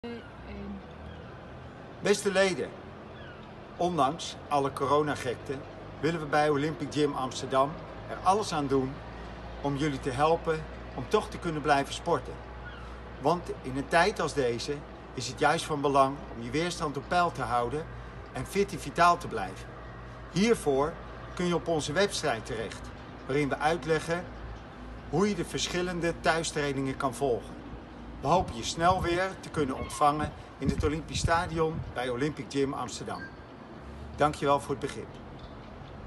Online Trainingen Olympic Gym Amsterdam vanaf heden te zien op ons YouTube kanaal en onze website!
Joost Hazes - Oprichter en hoofdtrainer Olympic Gym Amsterdam legt uit dat Olympic Gym er alles aan gaat doen om onze leden de gelegenheid te geven om vanuit thuis te blijven sporten. Juist nu is het belangrijk in beweging te blijven en zo jouw weerstand hoog te houden. Olympic Gym Amsterdam gaat de komende tijd via dit YouTube kanaal en onze website er alles aan doen om onze leden te voorzien van trainingen, workouts en content op gebied van voeding en weerstand.